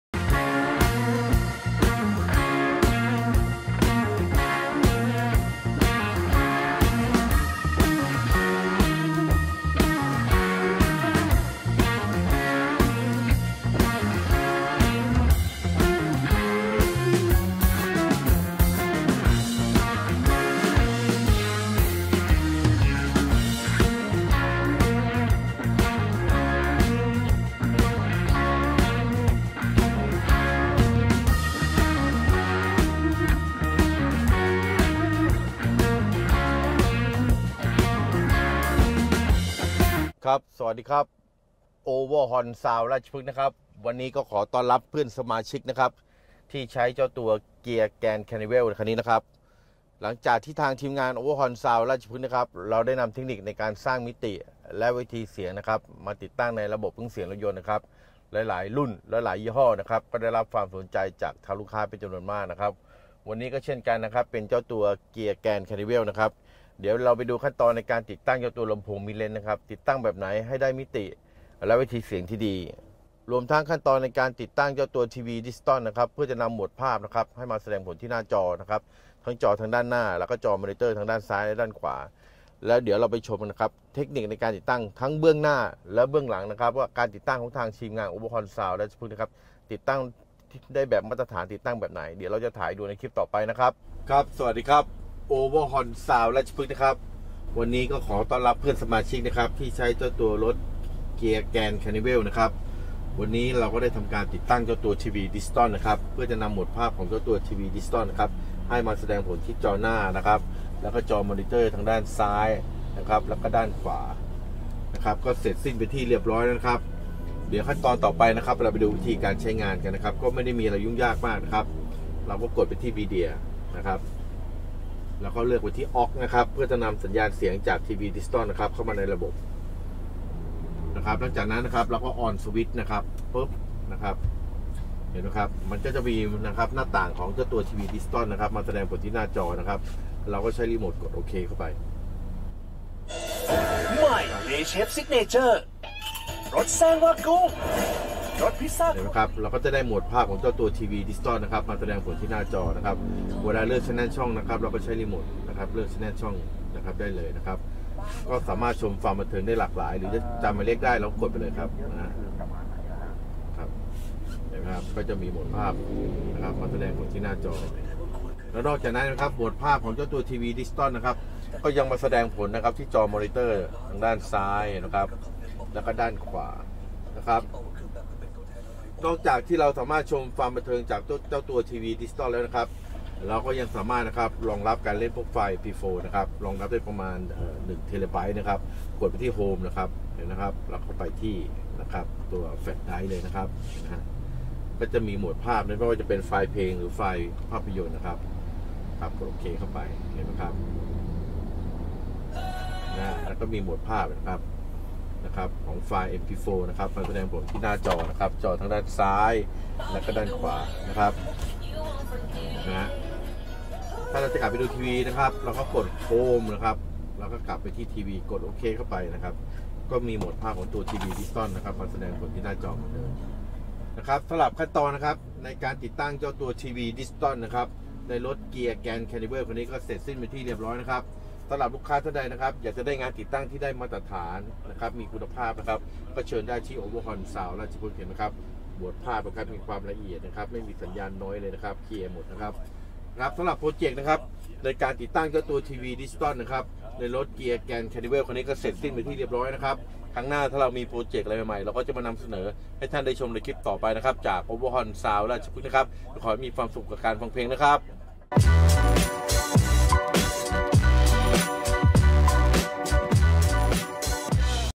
ปลดล็อคหน้าจอ KIA Carnival ดู TV Digital ,Youtube , Google Map พร้อมทั้ง 3 จอ
KIA CARNIVAL เป็นที่ยอมรับกัน จำนวนมาก กับเทคนิคของทางทีมงานโอเวอร์ฮอร์นซาวด์ ราชพฤกษ์ ที่สร้างมิติและเวทีเสียงที่ดี และในวันนี้จะทำการติดตั้งทีวีดิจิตอลเพื่อนำมาแสดงผลหน้าจอด้านหน้าและด้านหลัง การใช้งานสะดวกไม่ยุ่งยาก

โอเวอร์ฮอร์น ซาวด์ OverhornSound เครื่องเสียงรถยนต์ สาขาราชพฤกษ์ ปากเกร็ด นนทบุรี